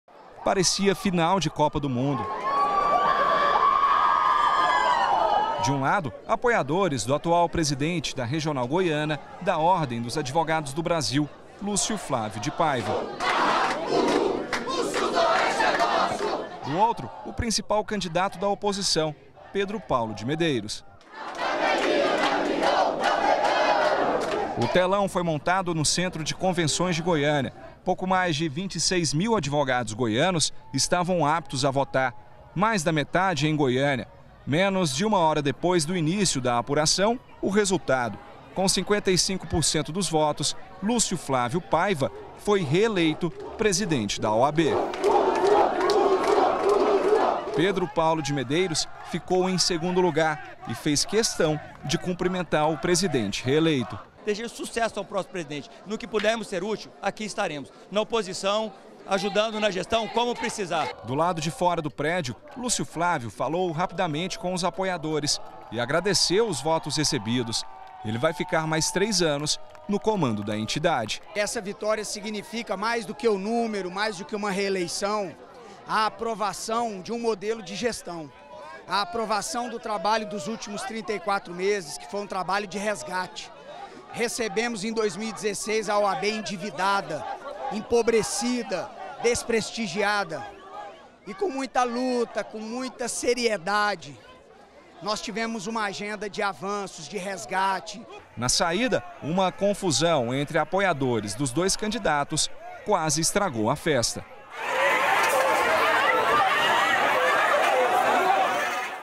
ELEIÇÃO NA OAB: LÚCIO FLÁVIO É REELEITO PRESIDENTE
O advogado Lúcio Flávio Paiva foi reeleito hoje a tarde presidente da OAB Goiás. ele recebeu 55% dos votos. A apuração aconteceu no centro de convenções, em clima de festa.